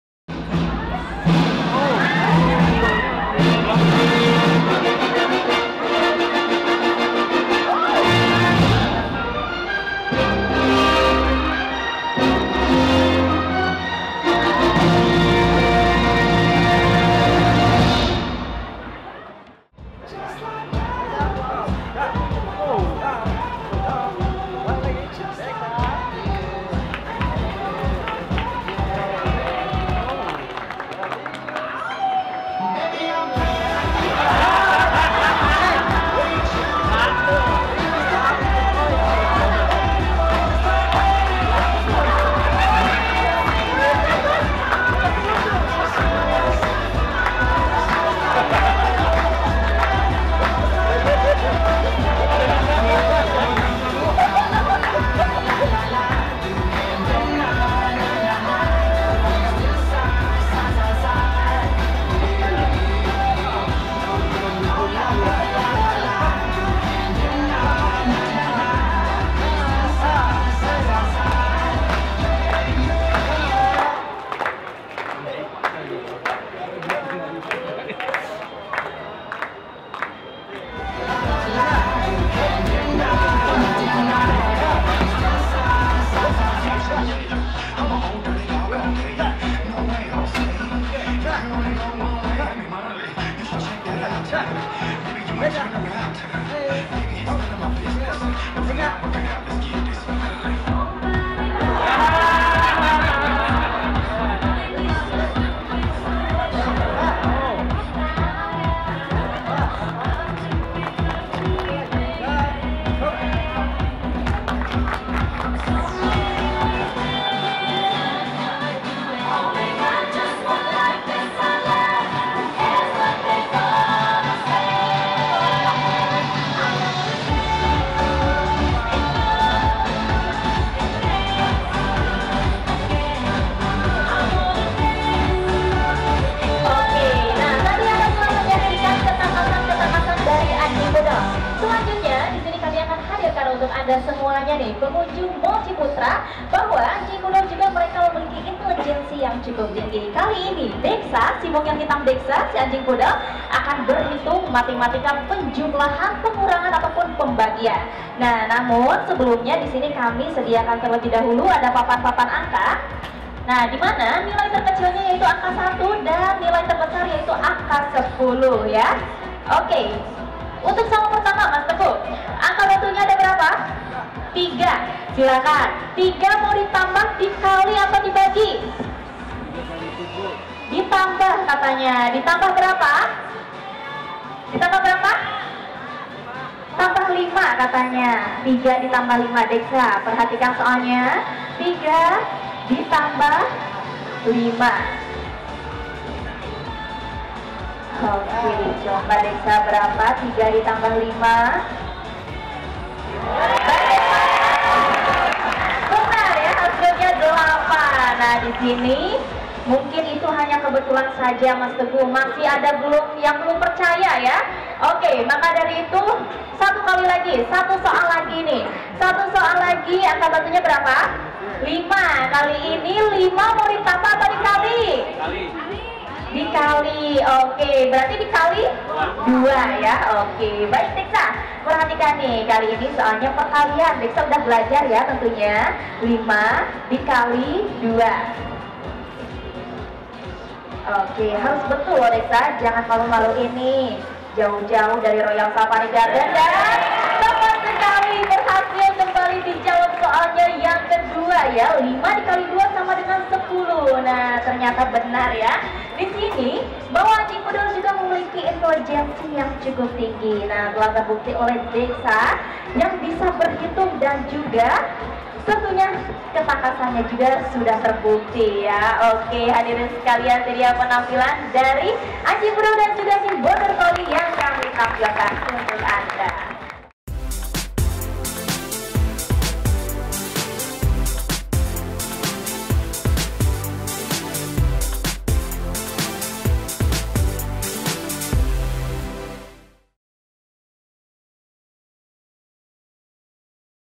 Dog Show Menyambut Imlek di Mall Ciputra - Bag 2
Beberapa waktu lalu, Bobo datang ke acara penyambutan tahun baru imlek yang diadakan oleh Mall Ciputra Jakarta. Di sana kita dapat menyaksikan pertunjukan atraksi anjing. Kita lihat keseruannya, Yuk!

Produser: David Togatorop
Editor : Sigit Purnomo
Videografer : Bambang Tetuko & Bonefasio Glent
Video Editor: Bambang Tetuko & Bonefasio Glent
Social Media Team : Sepdian Anindyajati